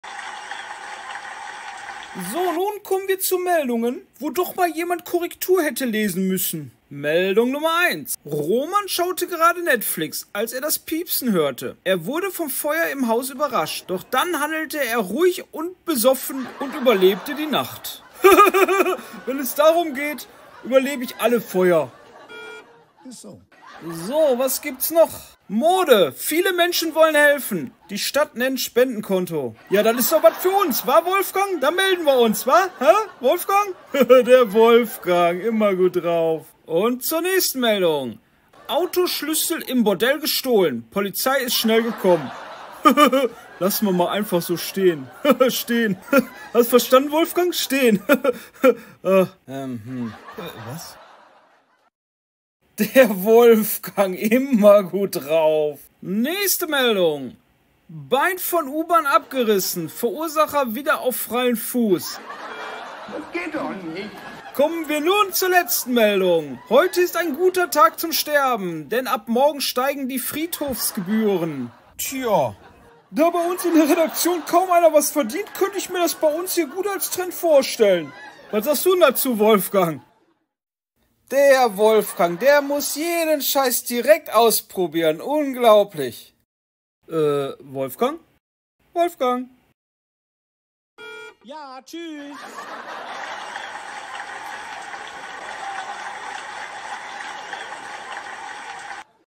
Tv Total : Die Verbotene und verloren geglaubte Folge, Parodie
Endlich die lange Verlorene Folge ist wieder aufgetaucht, na ja eigendlich ist nur ein überbrückungs Video bis zum nächsten Hörspiel, das etwas läger dauert als erwartet aber nun viel spass